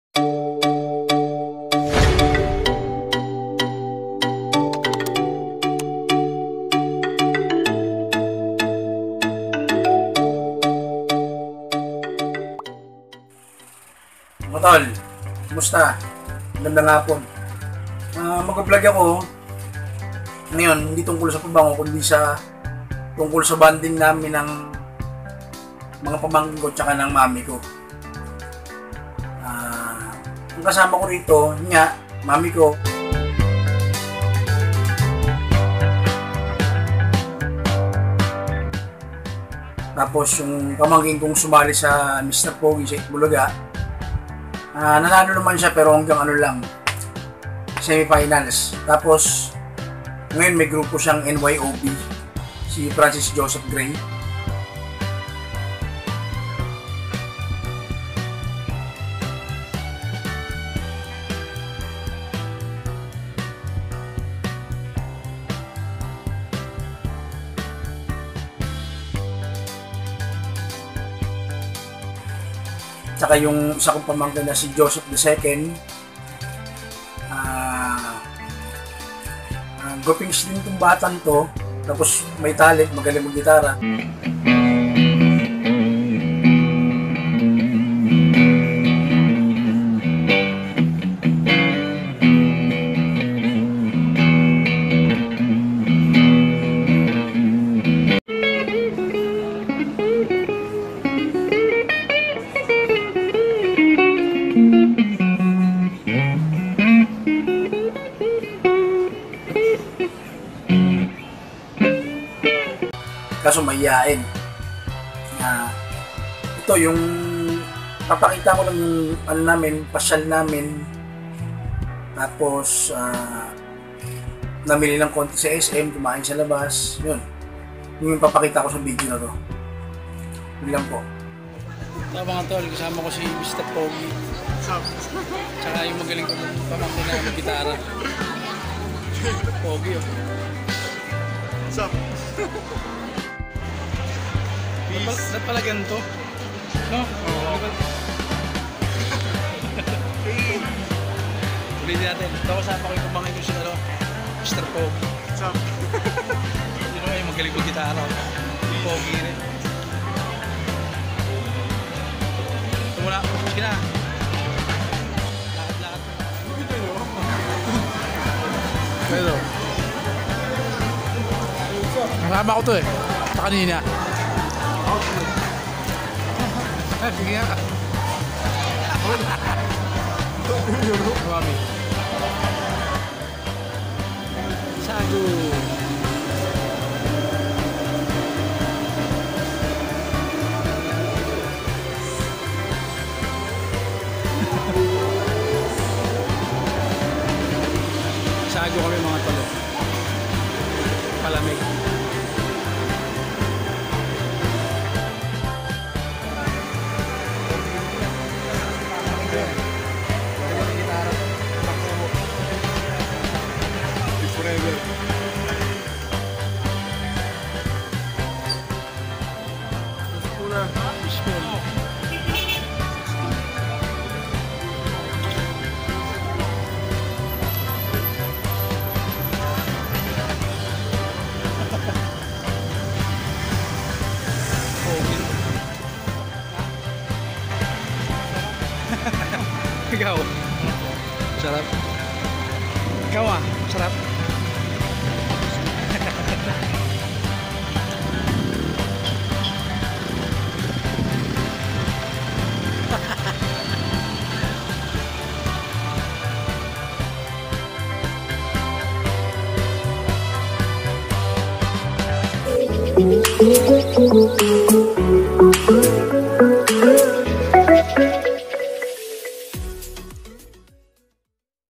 Vlog with Mike | Bonding with my beautiful mom and two handsome nephews
Hello mga ka-fam, today's video is all about sa galaan ni daddy Mike kasama ang maganda naming lola at dalawang pinsan na sina Joseph Grey II & Francis Joseph Grey.